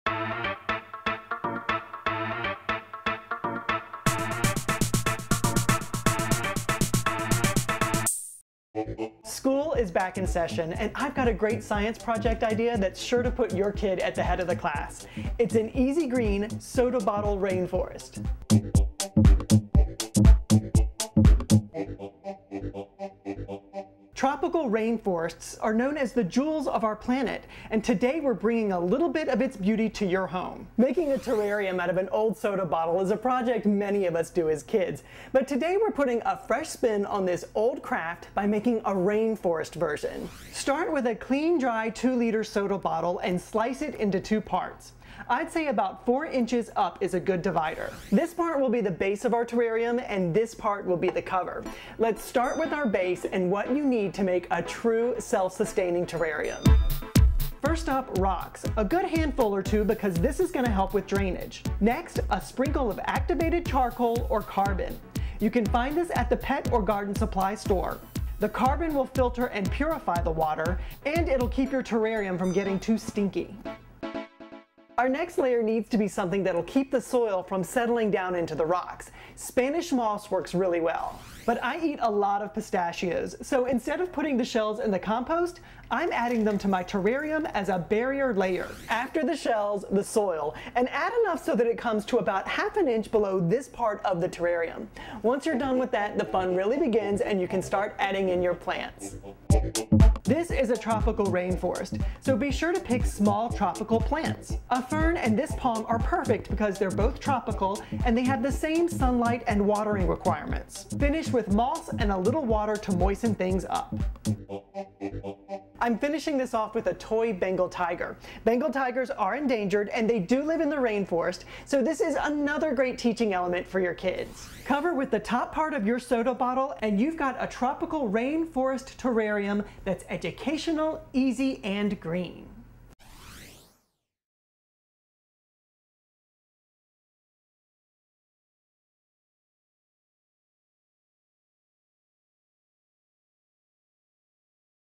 Rainforest in a Bottle
Building a terrarium out of a soda bottle is a project many of us do as kids.  But today we're putting a fresh spin on this old craft by making a RAINFOREST IN A BOTTLE.

I make awesome VEGAN recipes with an '80s vibe.